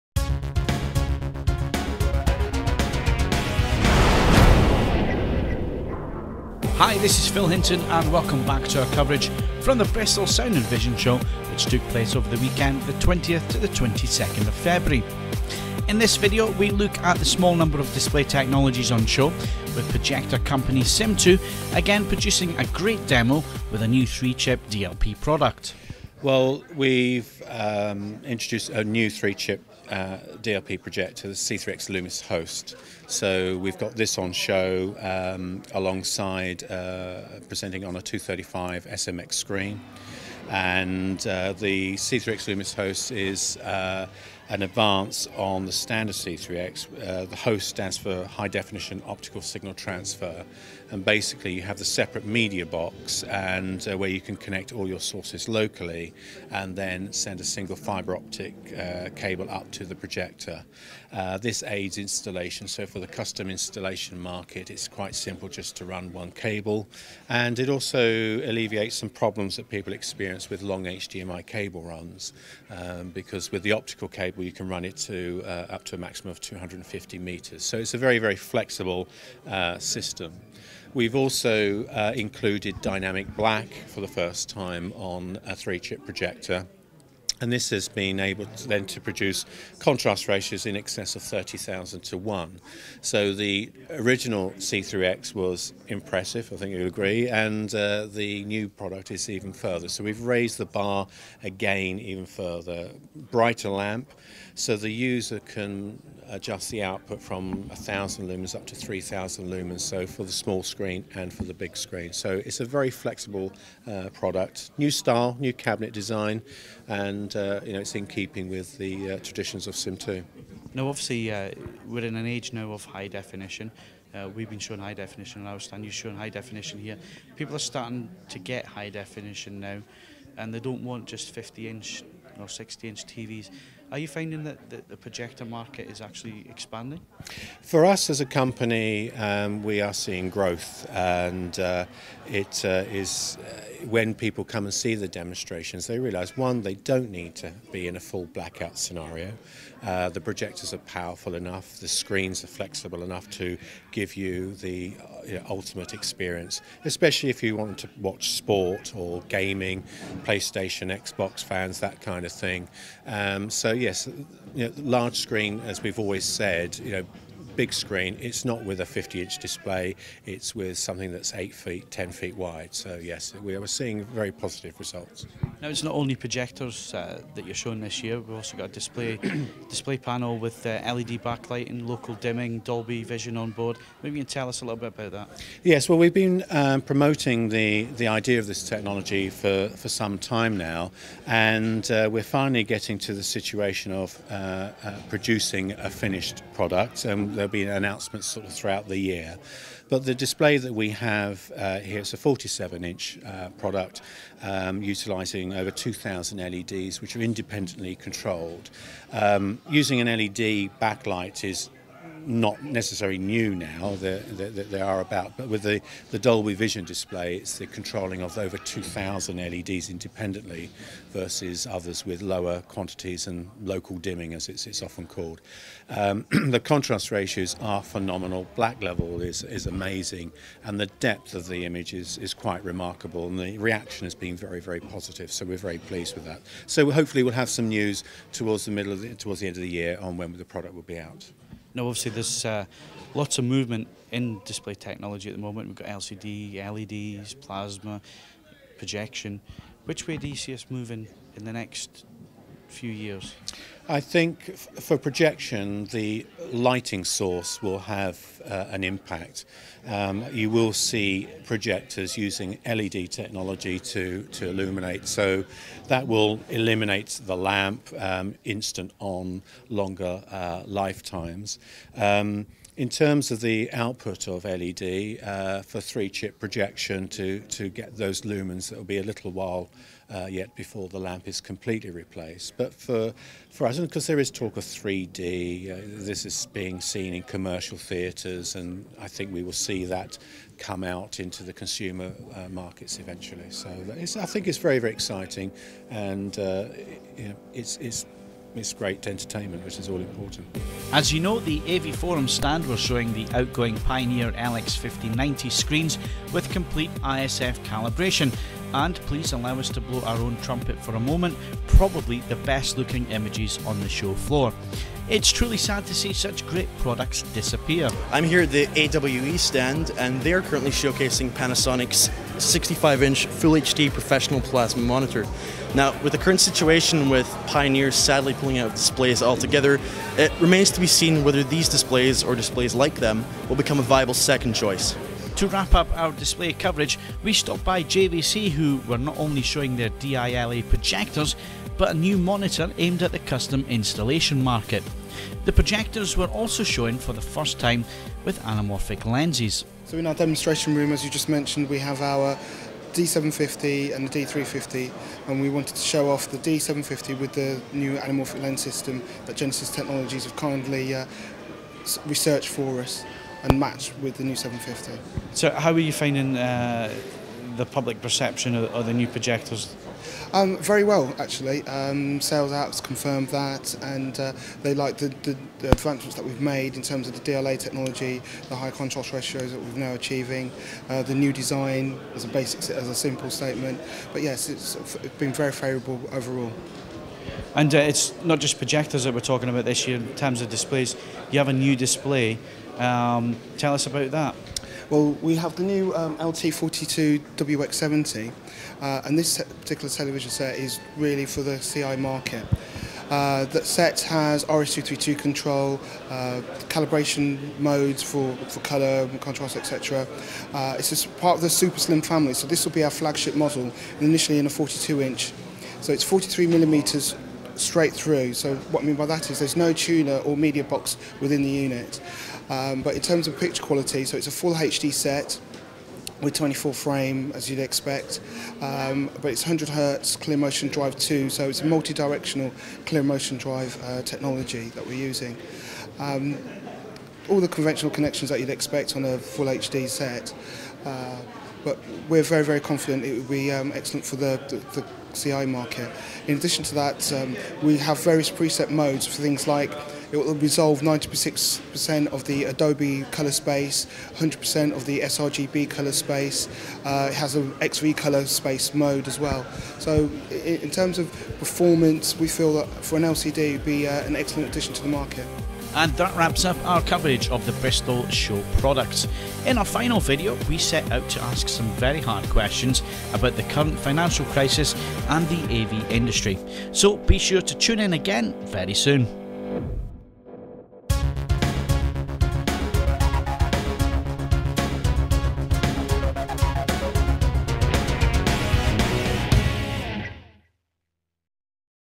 Bristol Sound and Vision show 2009 - Display Technologies
We take a look at a new 3 Chip DLP projector from Sim2. Hear about their Dolby Vision LCD TV, look back at Pioneer leaving the market, what Panasonic professional might offer in the future. And a new monitor for the CI market from JVC.